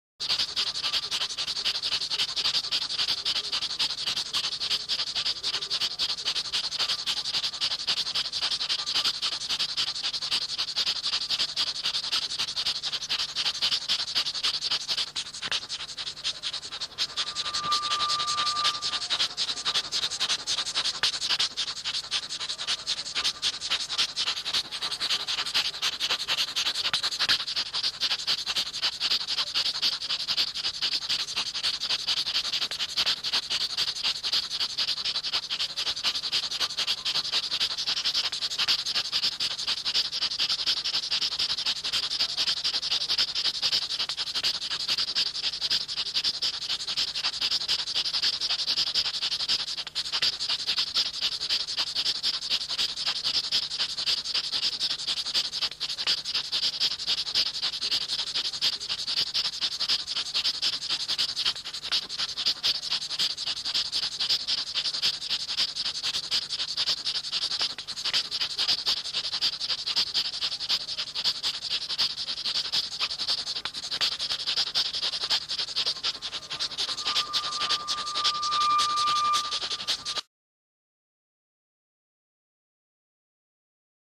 gorgrif_r1437impro
Hi !
My name is Gorgrif.
I'm artist and I need NOTHING to do my sounds.
I'm a kind of Human-Beat-Box and I create rythms with my mouth since a long time (1989).
I always do improvisations.
I recorded 89 cds full of improvisations between 1992 and 2007.
A huge work (2892 TRACKS).
Here you can listen the best I did!